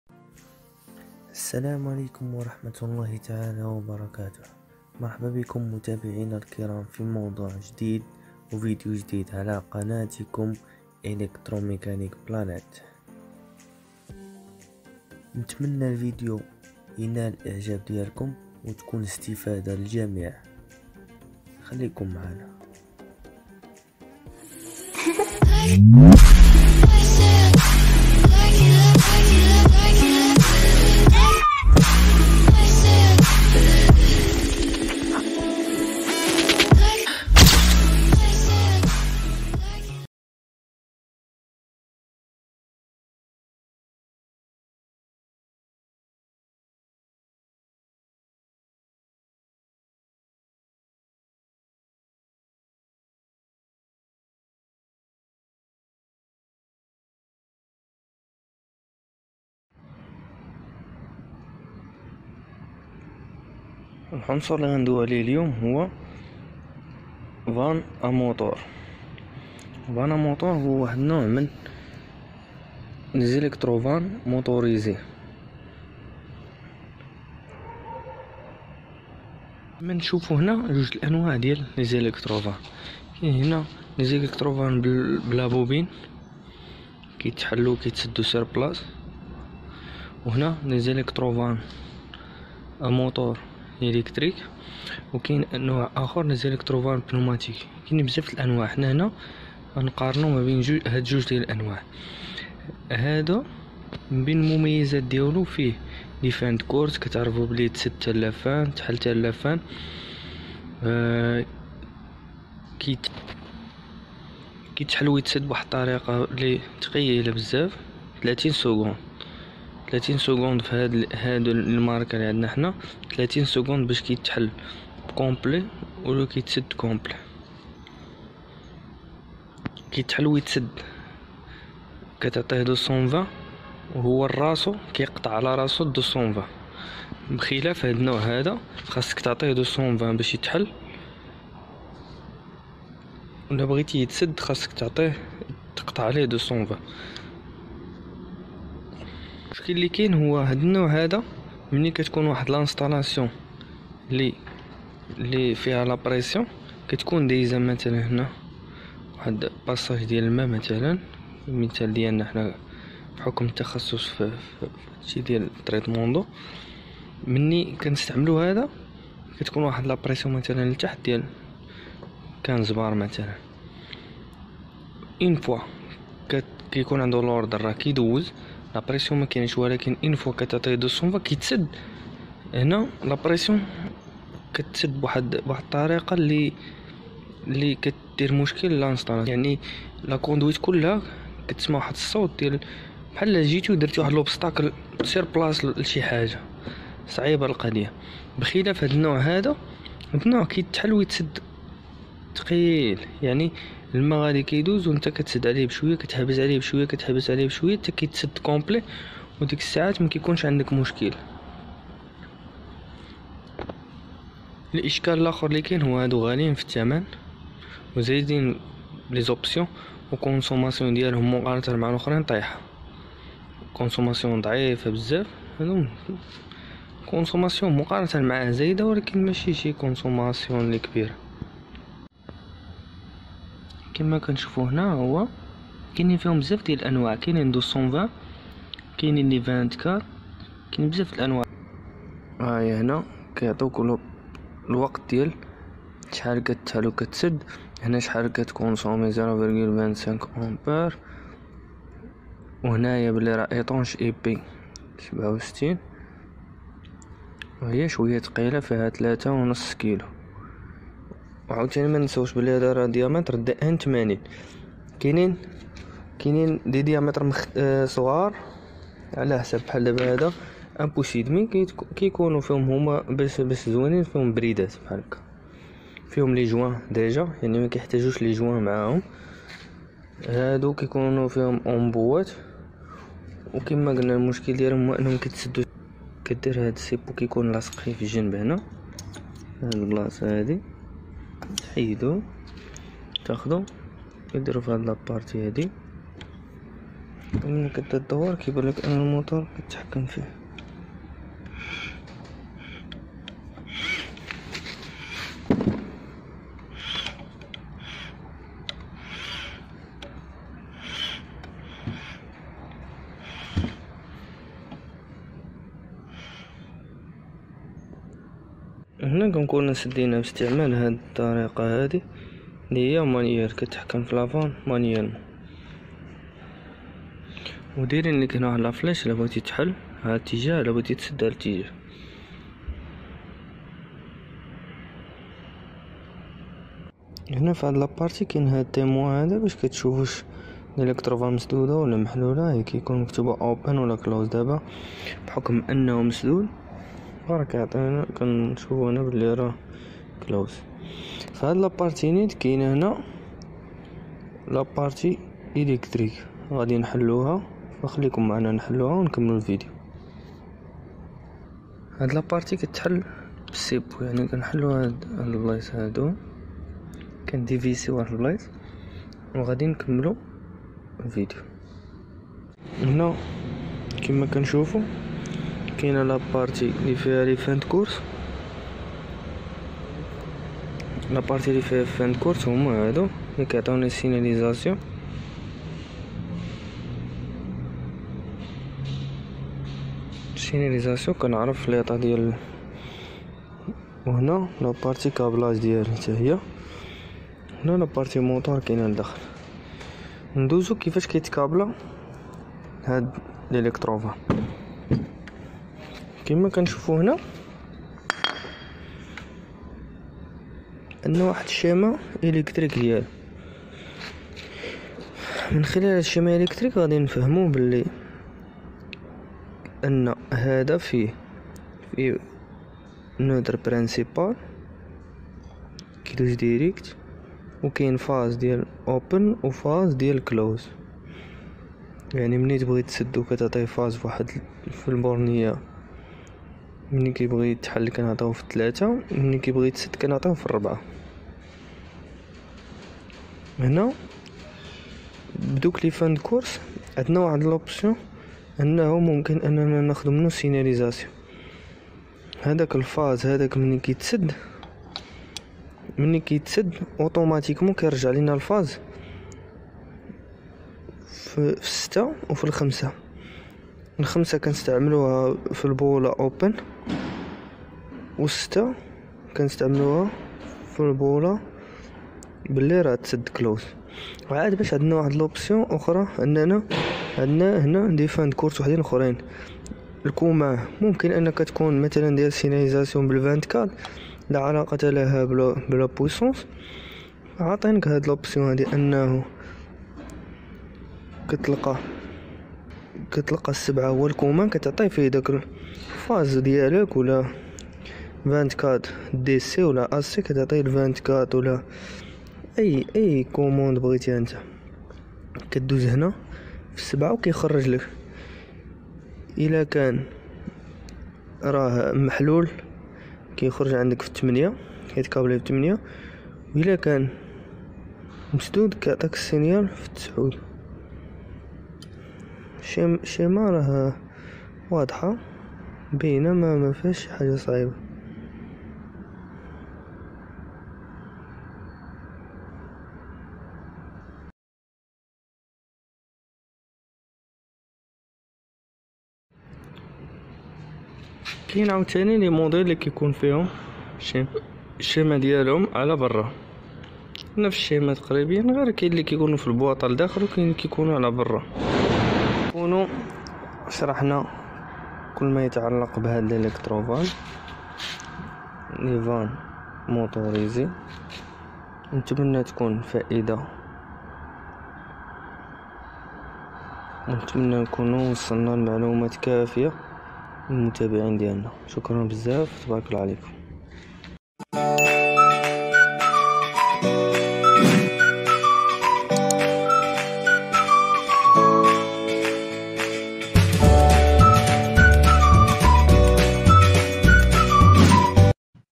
Vanne A Moteur
Présenté par :Mr . Adil Dagare et Mr. bouchaib Dagare
Pour plus d'information veuillez nous contacter sur:

installation
fonctionnement
déférence entre deux type de électrovanne
inconvénient et avantage entre les deux types